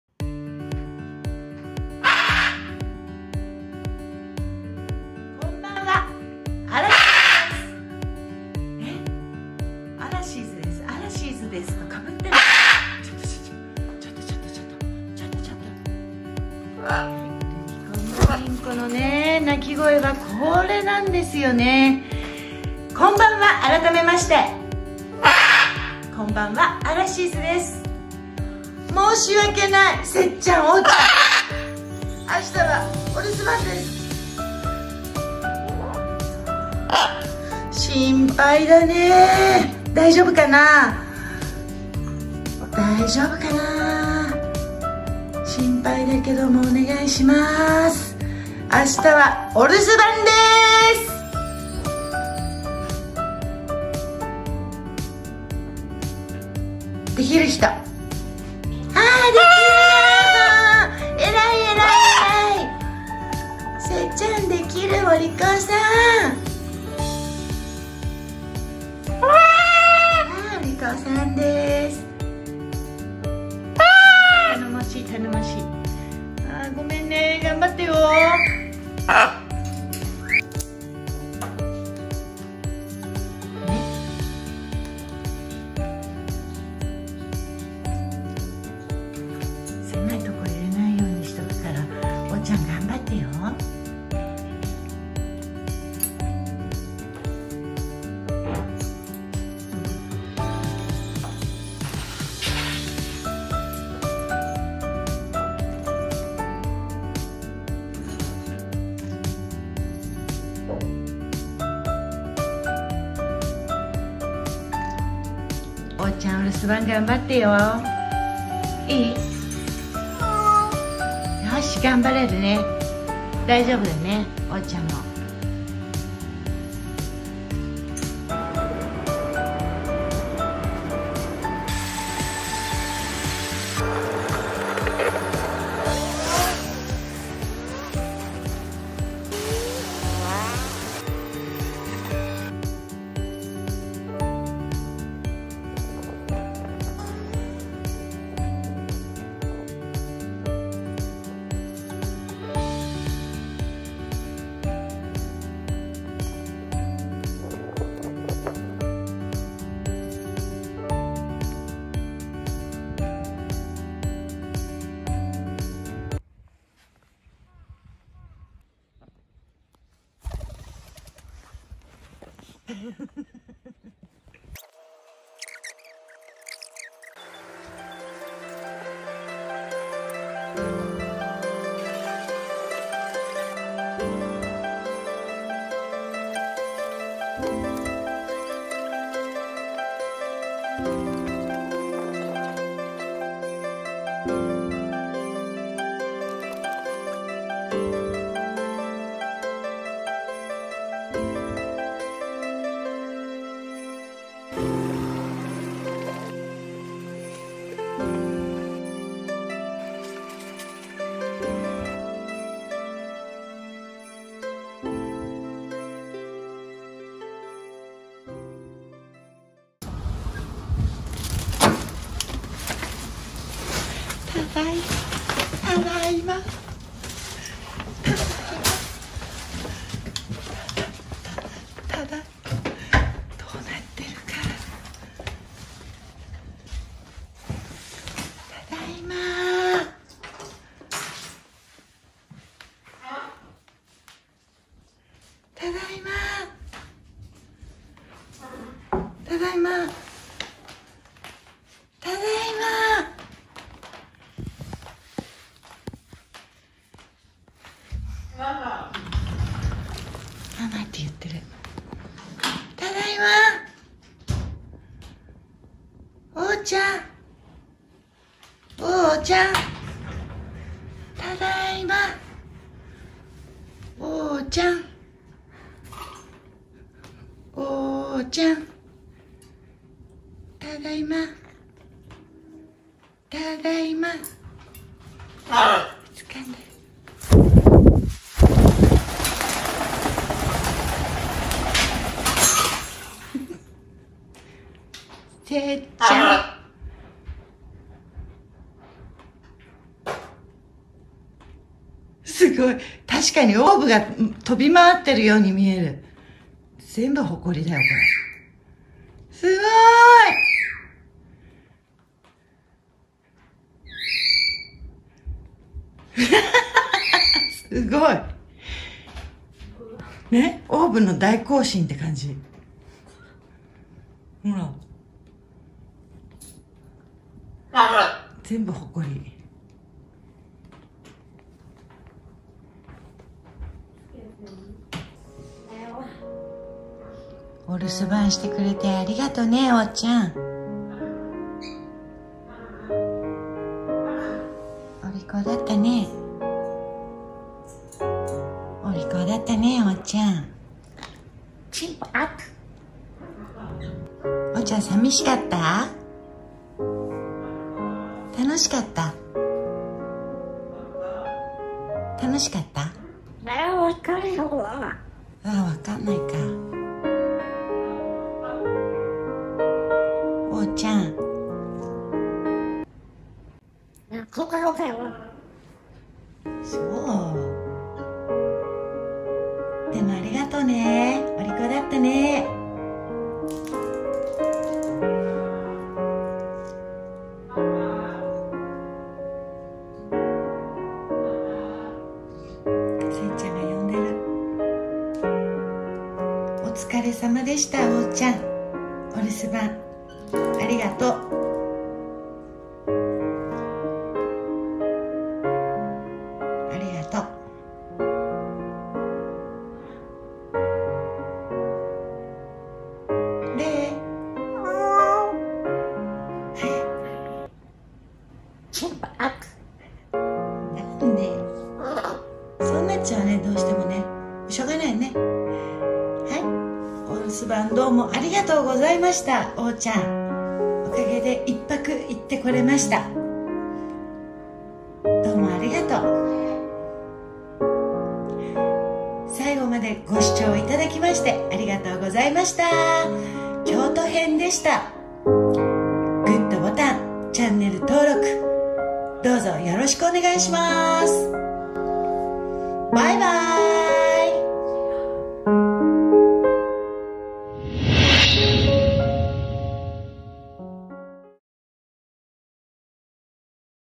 【亡き夫の鳥と生活】鳥ちゃんにお留守番交渉
旅行でペットをお留守番させるのは、本当に後ろ髪を引かれる思いです。
とくに寒い時期や夏は空調管理も気掛かりで旅先でも落ち着かないものです。
今回は親子で京都1泊旅を駆け足で過ごしました。